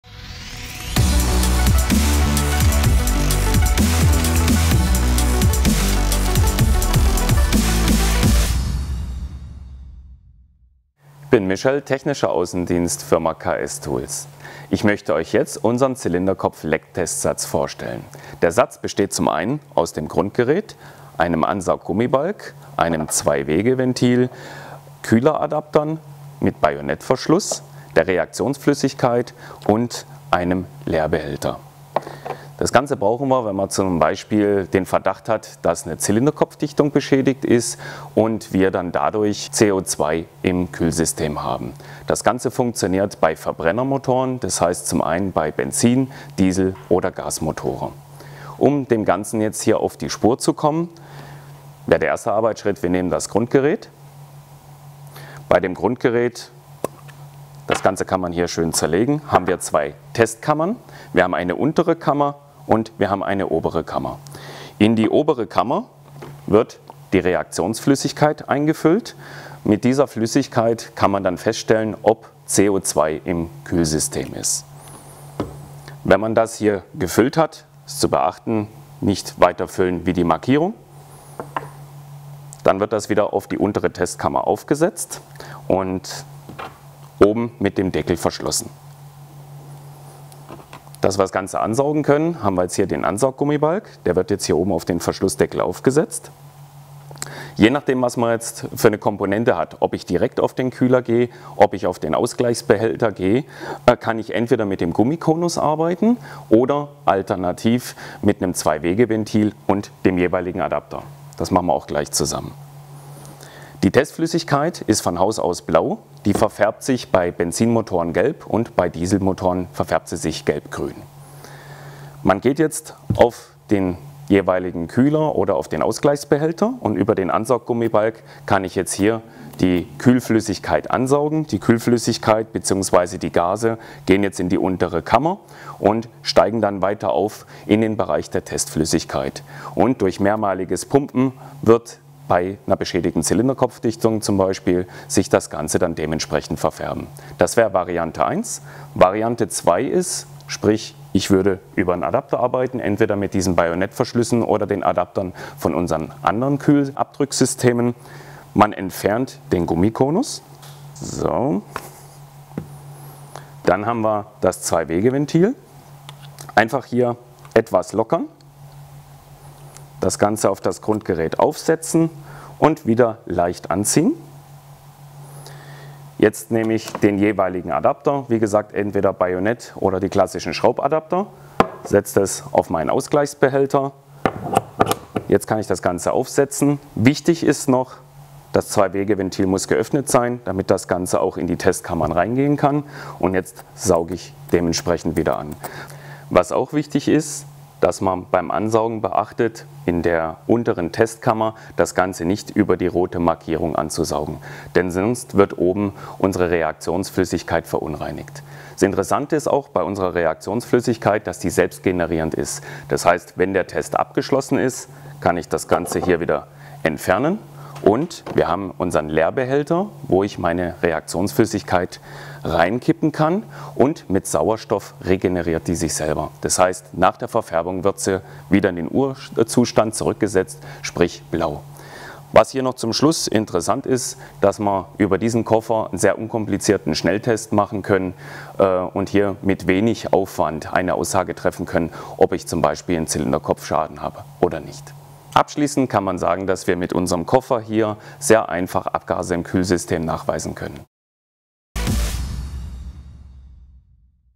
150.1910 CO2-Zylinderkopf-Lecktest-Satz 8-tlg.
• zur Lokalisierung von Undichtigkeiten an Verbrennungsmotoren
• rationelles und zuverlässiges Finden von Rissen und Leckagen
• eventueller CO2-Gehalt in der Kühlflüssigkeit wird nachgewiesen
• ideal einsetzbar bei Zylinderköpfen, Zylinderkopfdichtungen und
  Motorblöcken
• Undichtheit durch Verfärbung der Testflüssigkeit sichtbar
• für alle gängigen Fahrzeuge mit wassergekühlten Motoren (Gas,
  Diesel und Benzin)
• selbst regenerierende Reaktionsflüssigkeit für mehrfaches
  Verwenden
• in stabilem Kunststoffkoffer